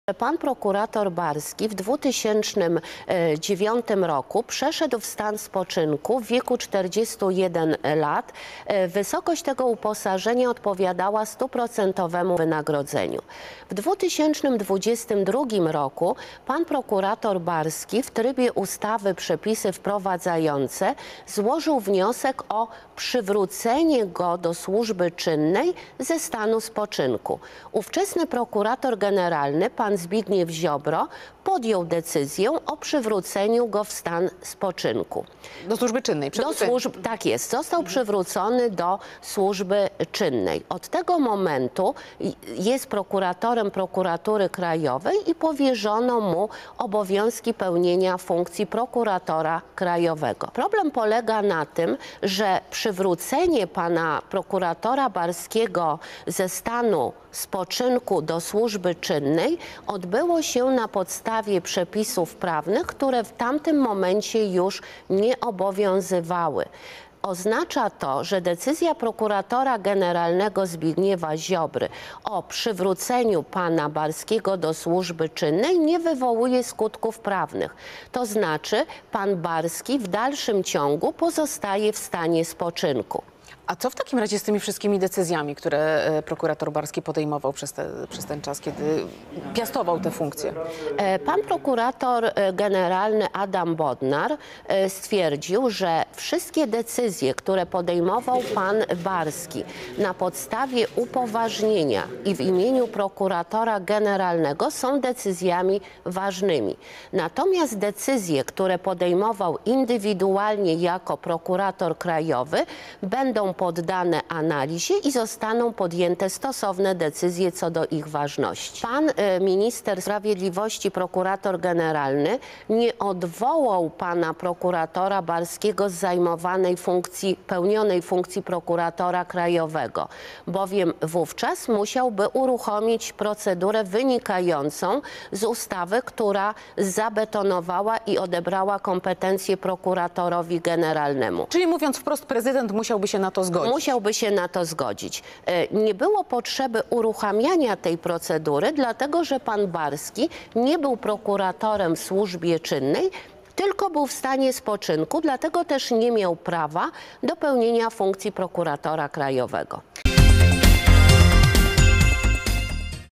Czy Dariusz Barski jest nadal prokuratorem krajowym?
Jak to z Barskim było. Klarownie, krok po kroku, wyjaśnia prokurator Katarzyna Kwiatkowska. Naprawdę warto poświęcić kilka chwilę.